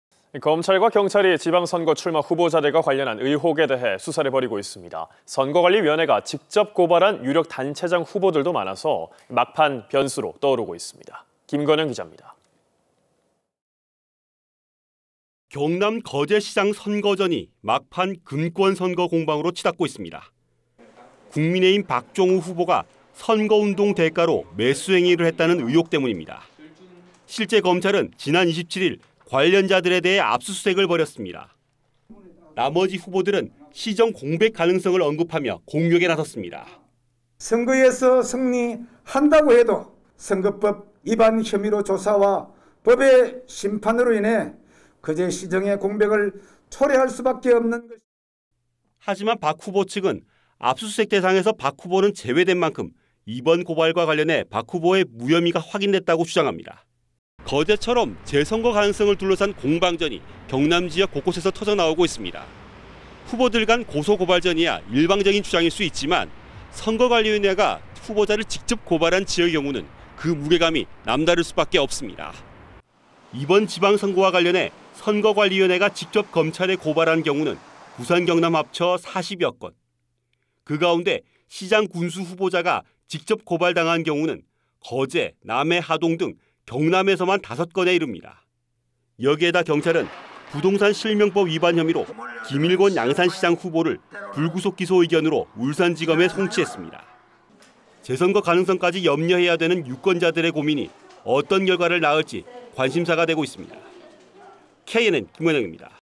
검경 수사·선관위 고발, 막판 변수되나
[앵커]
검찰과 경찰이 지방선거 출마 후보자들과 관련된 의혹에 대해 수사를 벌이고 있습니다.

선거관리위원회가 직접 고발한 유력 단체장 후보들도  많아서 막판 변수로 떠오르고 있습니다.

김건형 기자입니다.

[기자]
경남 거제시장 선거전이 막판 금권선거 공방으로 치닫고 있습니다.

국민의힘 박종우 후보가 선거운동 대가로 매수행위를 했다는 의혹 때문입니다.

실제 검찰은 지난 27일 관련자들에 대해 압수수색을 벌였습니다.

나머지 후보들은 시정공백 가능성을 언급하며 공격에 나섰습니다.

{김한표/무소속 거제시장 후보/"(국민의힘 시장 후보는) 선거에서 승리한다고 해도 선거법 위반 혐의 조사와 법의 심판으로 인해 거제시정의 공백을 초래할 수 밖에 없습니다."}

하지만 박 후보측은 압수수색 대상에서 박 후보는 제외된만큼 이번 고발과 관련해 박 후보의 무혐의가 확인됐다고 주장합니다.

"거제시장 선거처럼 재선거 가능성을 둘러싼 후보들간 공방이 경남 곳곳에서 터져 나오고 있습니다.

후보들간 고소,고발전이야 일방적 주장일 수도 있지만 선거관리위원회가 직접 후보자를 고발한 지역들의 경우는 그 무게감이 다를 수 밖에 없습니다."

이번 지방선거와 관련해 선거관리위원회가 직접 검찰에 고발한 경우는 부산,경남 합쳐 40여건.

그 가운데 시장,군수 후보자가 직접 고발당한 경우는 거제, 남해, 하동 등 경남에서만 5건에 이릅니다.

여기에다 경찰은 부동산 실명법 위반 혐의로 김일권 양산시장 후보를 불구속 기소 의견으로 울산지검에 송치했습니다.

재선거 가능성까지 염려해야 되는 유권자들의 고민이 어떤 결과를 나을지 관심사가 되고 있습니다.

KNN 김건형입니다.